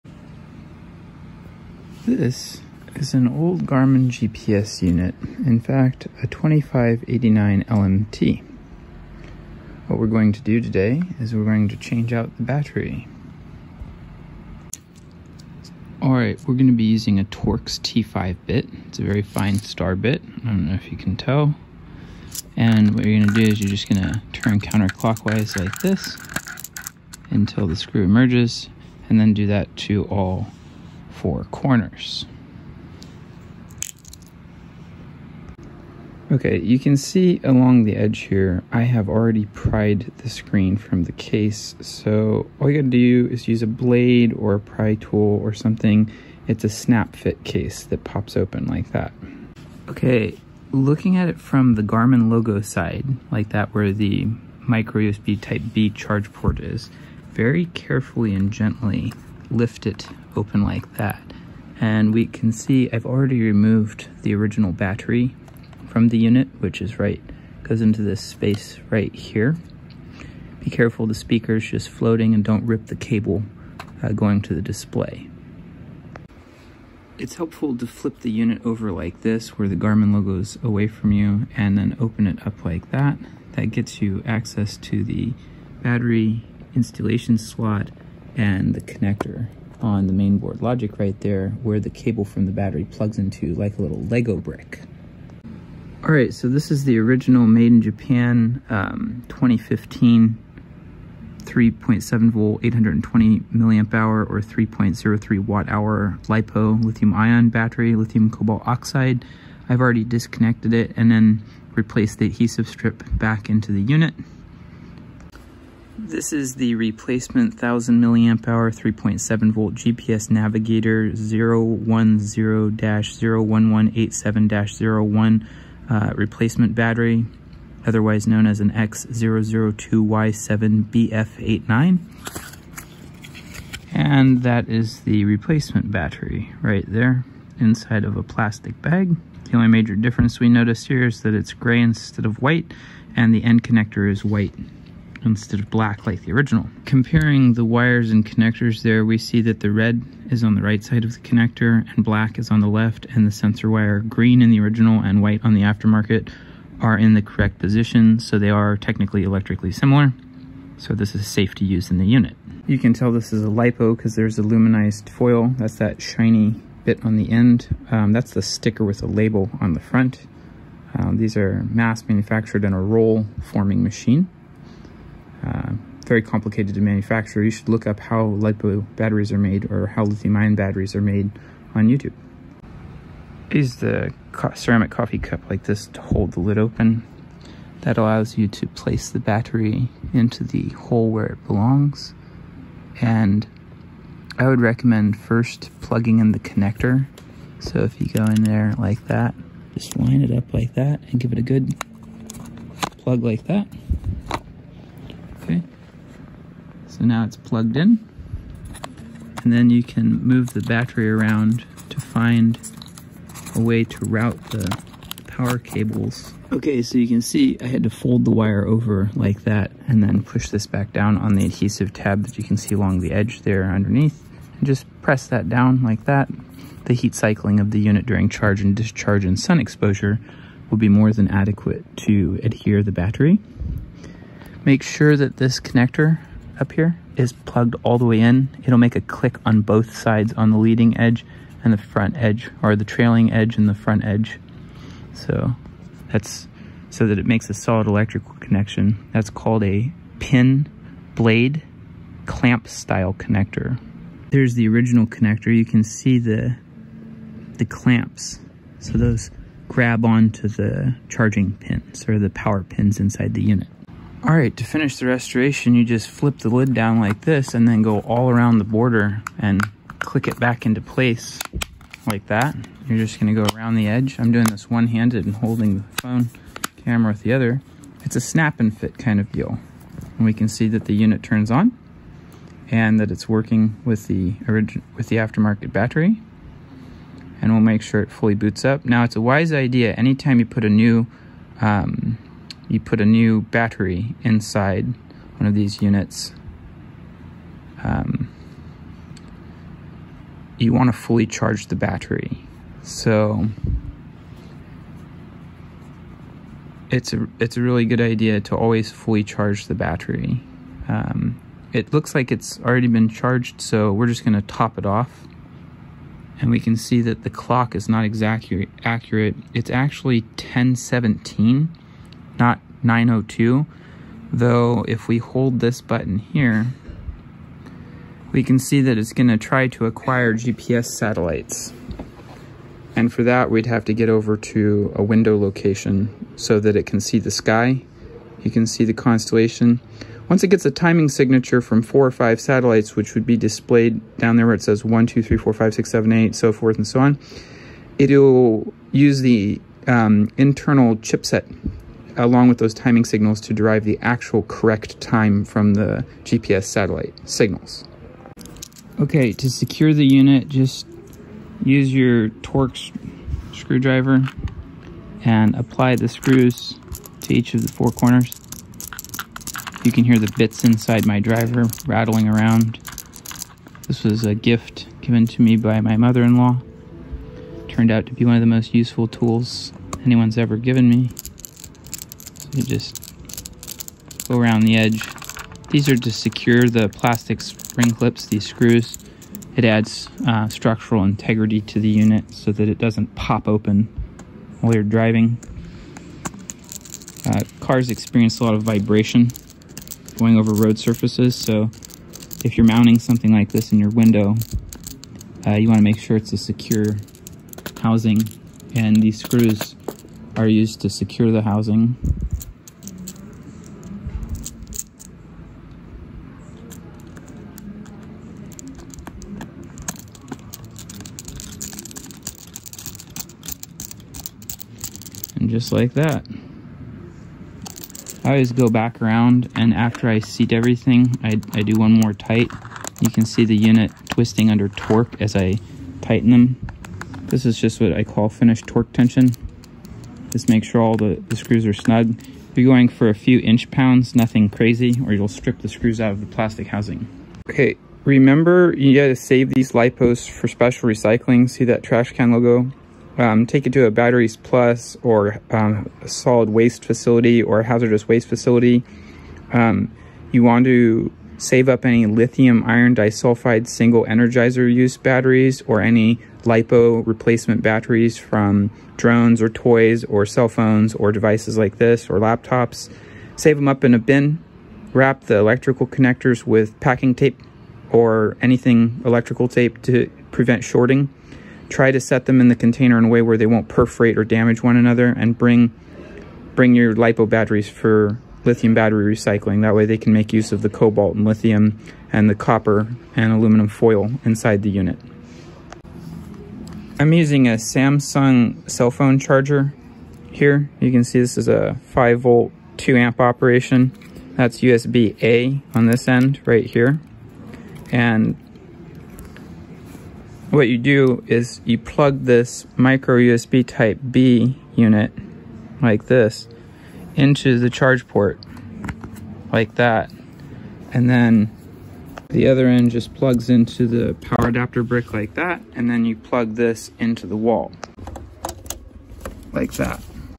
Garmin NUVI 2589LMT Battery Change DIY "How To" Video
Here, I hold my phone camera (iPhone SE2) with one hand & use the tools in the other, more or less to give you a quick tutorial or How To DIY change your faded or weak Garmin Battery.

The procedure depicted here broadly applies to other GPS models. You go online & look up the model number printed on the back of your unit with the words "replacement battery" & order a replacement battery, then use a tech repair kit with special tools like the T5 star security bit for the screws on all four corners of this Garmin NUVI 2589 LMT GPS navigation unit, to open it.

One the unit open, take care to fold it open so as to not rip the screen ribbon cable. Carefully disconnect the battery connector from the mainboard logic, then gently pry up the OEM battery without damaging the battery (otherwise it could catch fire, vent with flame & make a bunch of nasty cobalt rich toxic blue grey stinky smoke)(I know from working on iPhones where someone else glued a replacement battery in with epoxy, as a WISE level 2 professional device repair technician)(where that actually happened)

This Garmin NUVI 2589LMT from 2015, & the software last updated with 2018 revision maps & POI's or points of interest. I keep this as backup in case my cell phone stops working & I need to GTFO & take a route I am not familiar with to a safe point or remote secure spot. Sometimes during natural hazards & disaster, the cell phone networks are either not working or overloaded, so unstable. This kinda in the prep-er spectrum of being prepared for unforeseen future events, hazards & disasters.

The 4K video edited in iMovie & exported as a .mp4 file, then extracted for a Thumbnail image & then uploaded directly to YouTube. The commentary not a voice over, I was just talking while one handed handheld recording the video clips. I had already pried the unit open about a week ago to get the battery model data so I could order the correct replacement battery on Amazon for ~$10 USD2022 as it were //

This video roughly documents my finishing that process! I share it with you as a crude tutorial in the spirit of DIY device repair at home! I believe that fixing electronics by replacing the lipo battery ethical if the only thing wrong with the unit a weak faded lithium polymer battery, which as a battery technology tend to fade badly or lose their ability to hold a useful amount of charge, after a few years, or badly faded after 5 years in this example. It was kept in the center console of my Prius II where it experienced a lot of thermal cycling, which makes the LIPO battery in the Garmin degrade faster.

I recommend watch Louis Rossman Device Repair videos on YouTube
if you are interested in Device Repair laws or fixing stuff that you already own instead always buying a new one, especially when your old one works just fine & the screen is cracked or the battery faded, so fixing it much cheaper than replacing the whole thing. I got LMT or lifetime maps & traffic with the intent of keeping this unit for a long time! That means I continue to get radio traffic data & can use my windows laptop with the GARMIN update app to download map updates to this unit for years, many years. A new update is actually available right now & I will be updating the software soon.